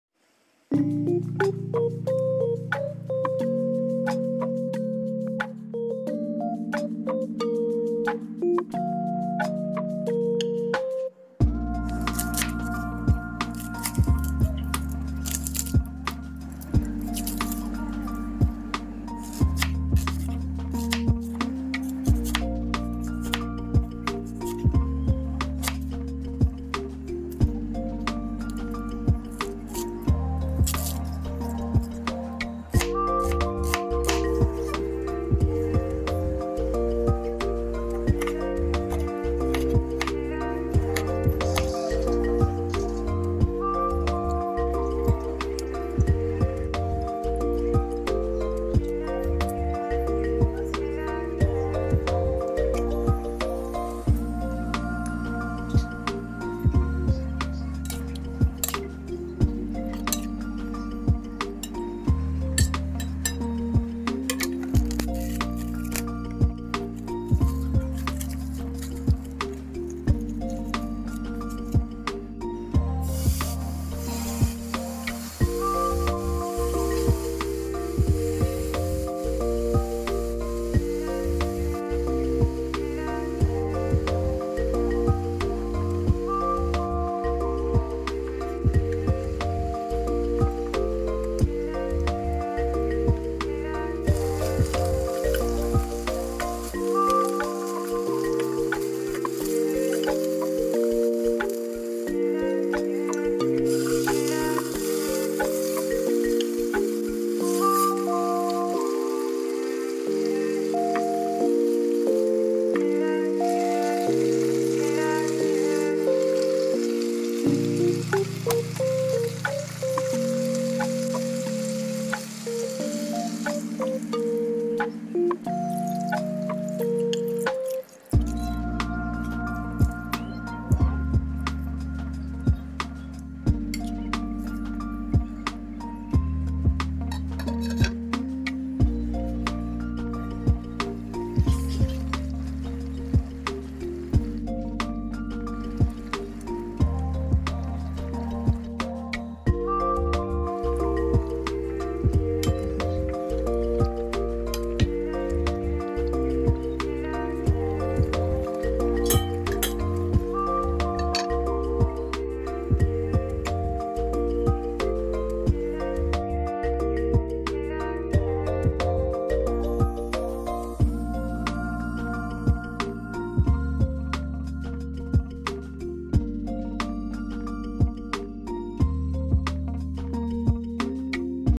PORK BISTEK (MARINATED PORK STEAK)
Here's the easiest and quick Pork Bistek na magugustuhan ng buong family. Less oil ang recipe na ito. Try na! 😋